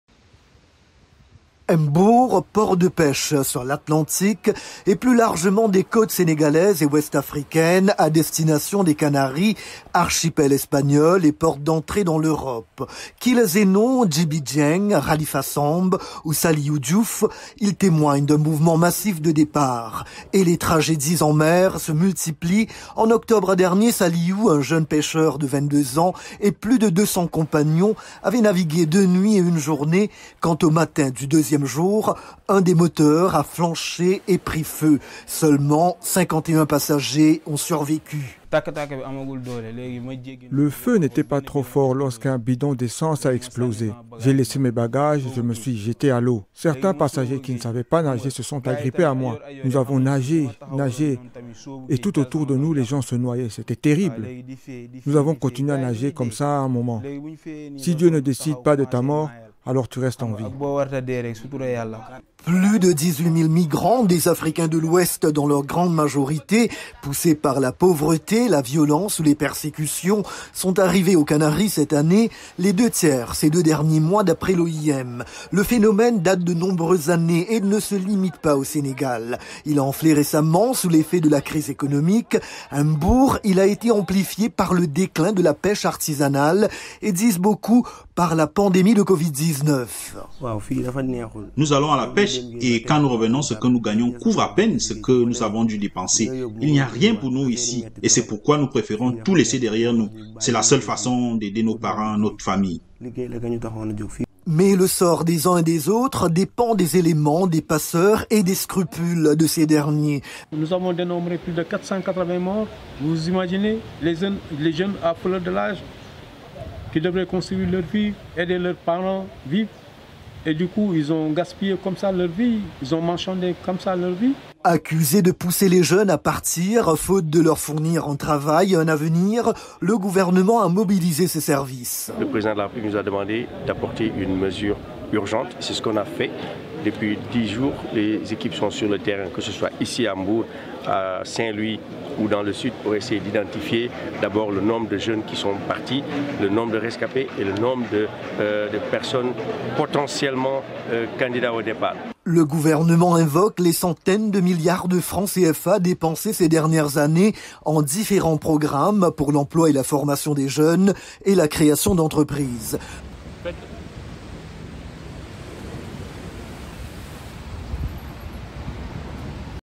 Émigration: Les tragédies continuent au large des côtes de sénégalaises
L'un raconte la soif, un autre la tempête, un autre encore le naufrage qui ont anéanti leur rêve d'Europe quelque part entre l'Afrique et les Canaries. Malgré les épreuves et l'échec, ces jeunes pêcheurs sénégalais disent qu'ils repartiront à la première occasion. Plus de 500 personnes ont perdu la vie cette année, la plupart en octobre et novembre, sur ce qui est l'autre route maritime de l'Europe, avec la Méditerranée, selon l'Organisation internationale pour les migrations (OIM). C'est une estimation basse mais c'est déjà deux fois plus qu'en 2019.